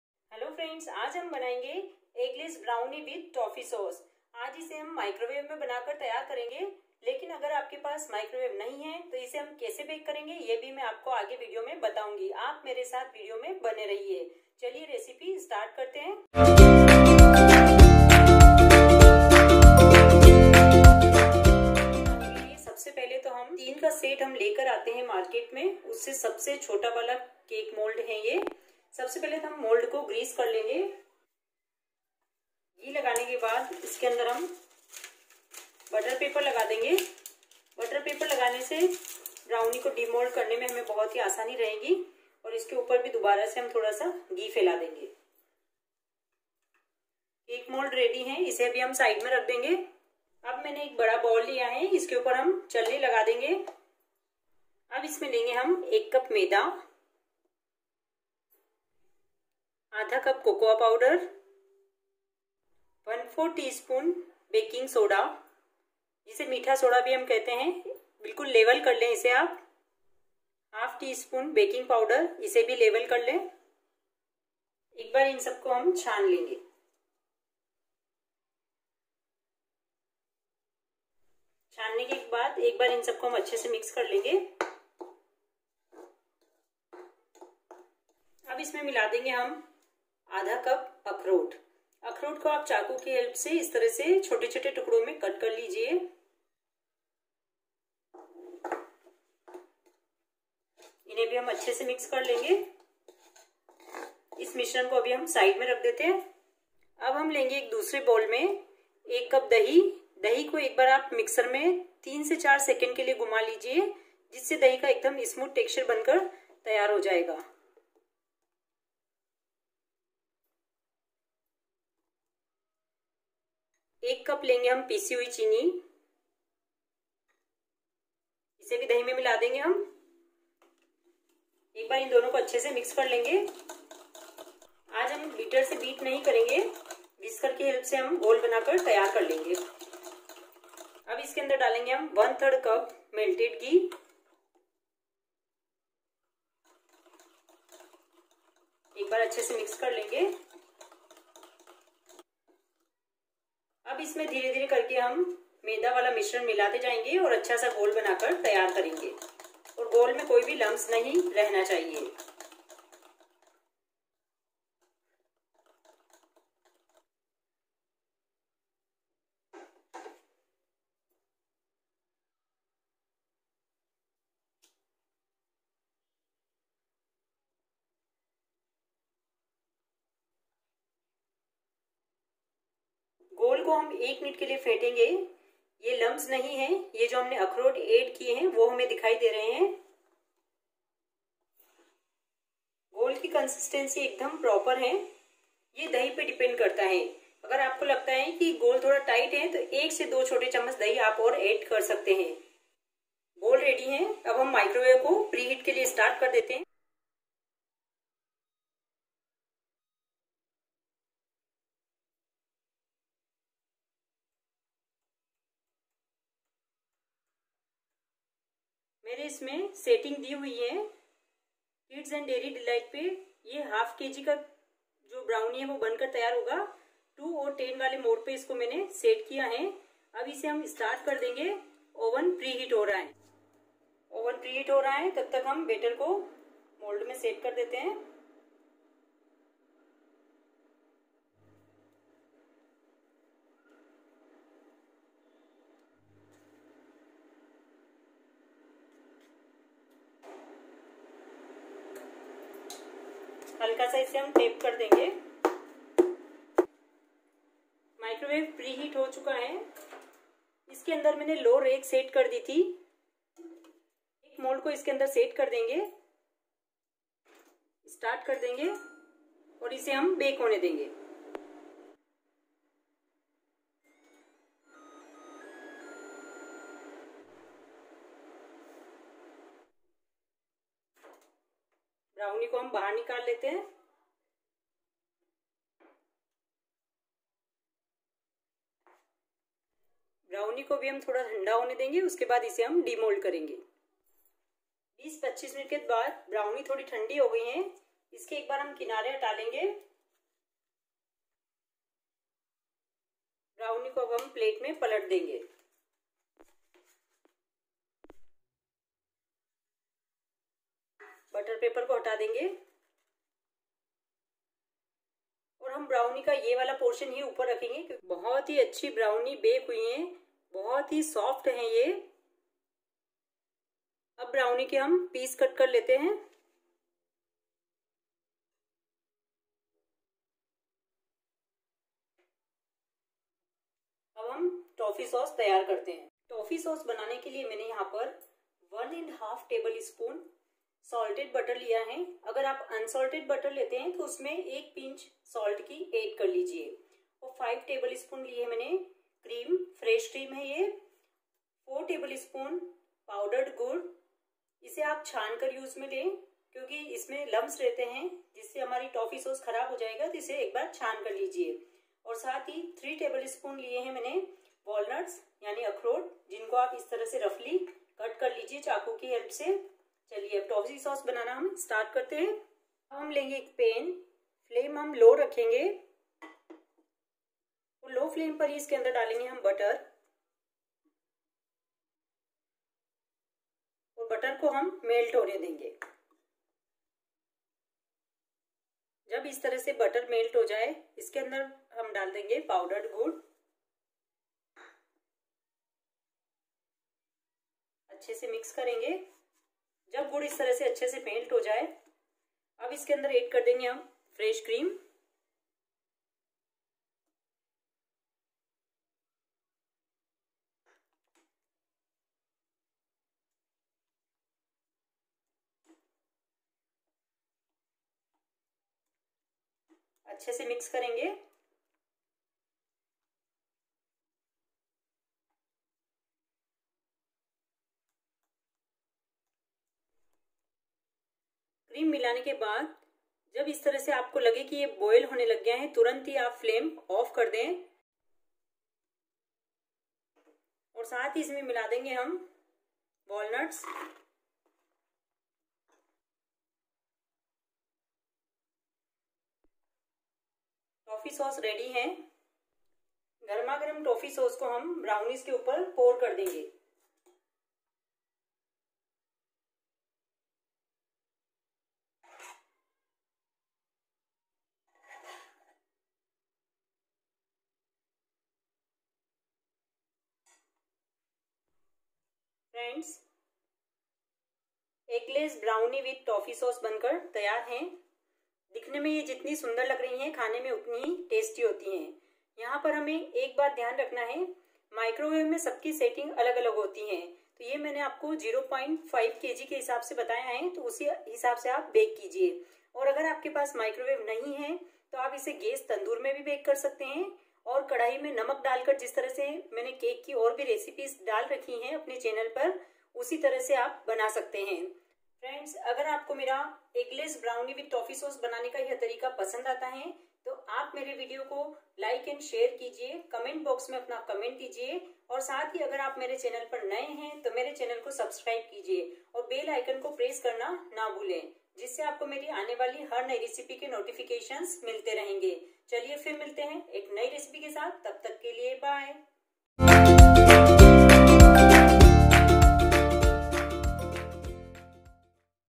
Eggless Brownie with Toffee sauce | Eggless Brownie recipe in microwave or without microwave
Hello friends,
Today I am presenting to you how to make eggless brownie recipe with toffee sauce it is very soft and spongy and its taste is very delicious. today we are bake it in microwave and tips for without microwave. Let's try and enjoy this brownie with your family.

INGREDIENTS -
FOR BROWNIE -
1. All purpose flour (maida) - 1 Cup
2. Cocoa powder - 1/2 Cup
3. Walnuts - 1/2 Cup (chopped)
4. Fresh curd - 1 Cup
5. Powdered sugar - 1 Cup
6. Baking soda - 1/4 tsp.(leveled)
7. Baking powder - 1/2 tsp.
8. Melted ghee - 1/3 Cup

FOR TOFFEE SAUCE -
1. Butter - 1&1/2 tbsp.(softened)
2. Powdered jaggery - 4 tbsp.
3. Fresh cream - 5 tbsp.
4. Walnuts - 2 tbsp.(roughly chopped